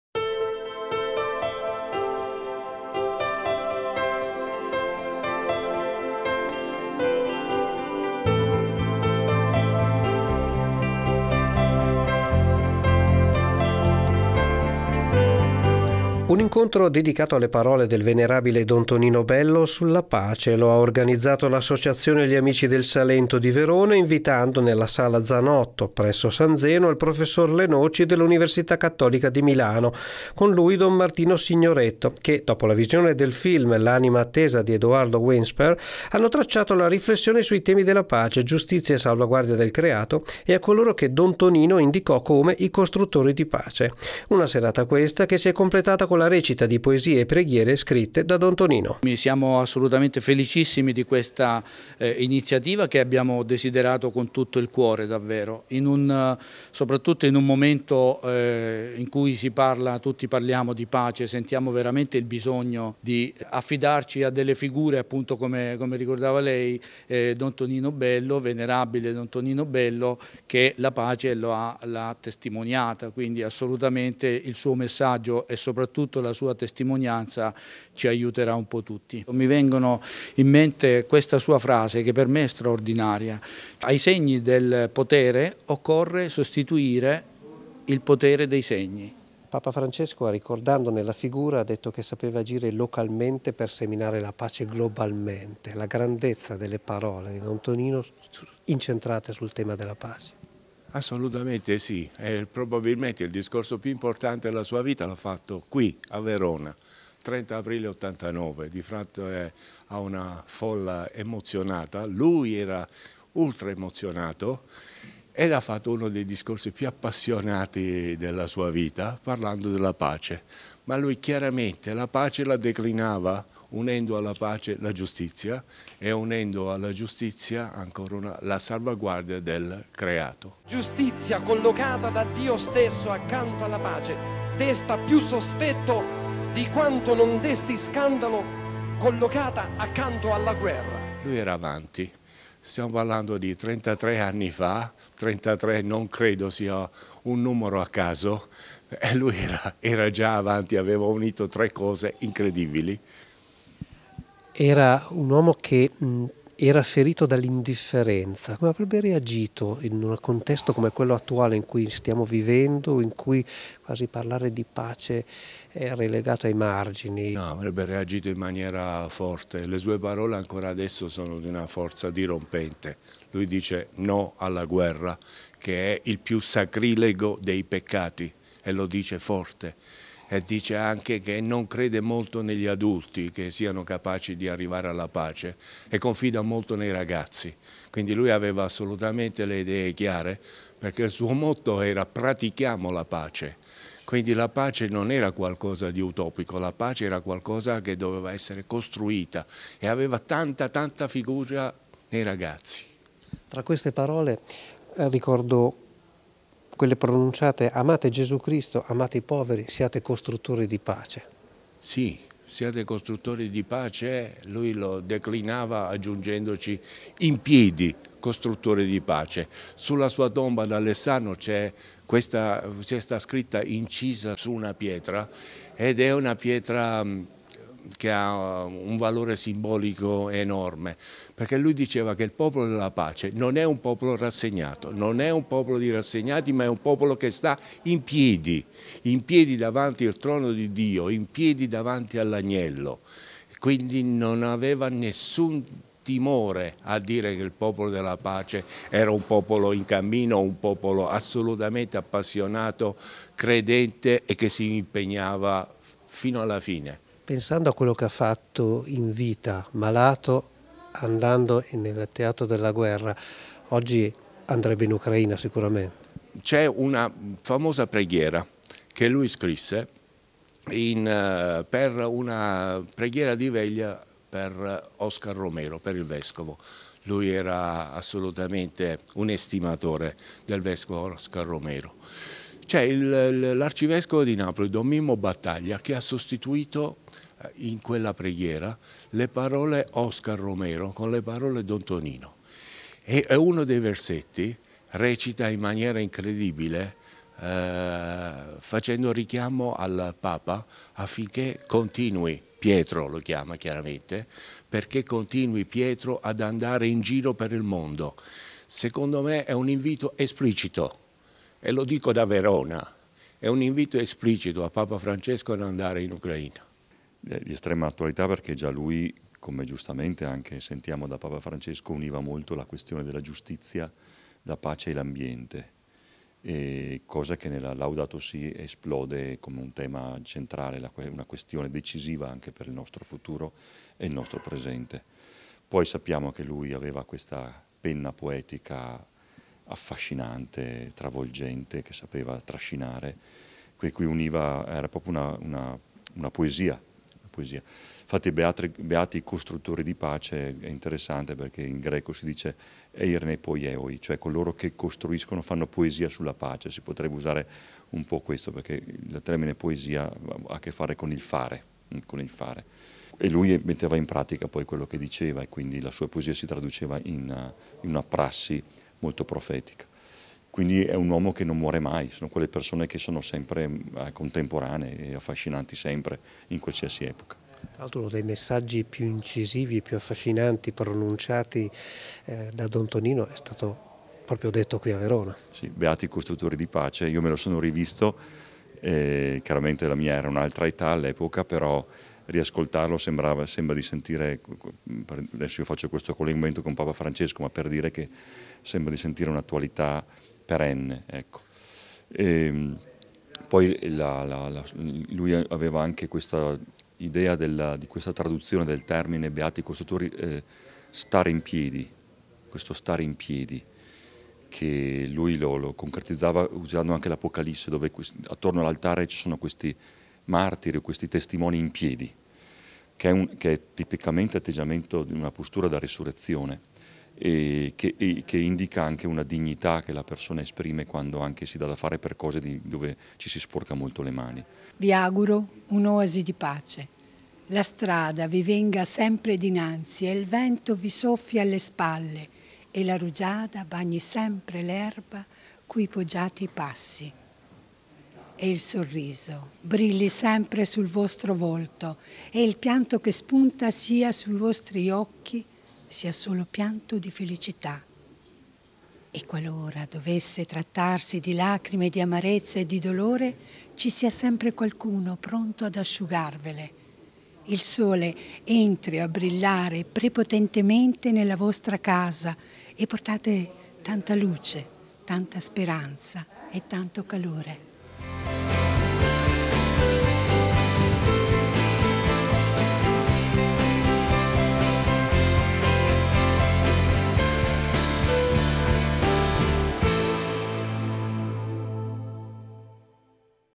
L'approfondimento - Don Tonino Bello e la pace
Verona ha ospitato, ieri sera, nella sala Zanotto nei pressi della Basilica di San Zeno, un incontro dedicato alle parole del venerabile Don Tonino Bello e la pace. L'iniziativa, organizzata dall'associazione "gli amici del Salento" ha visto la partecipazione di Don Martino Signoretto e del professor Francesco Leonci.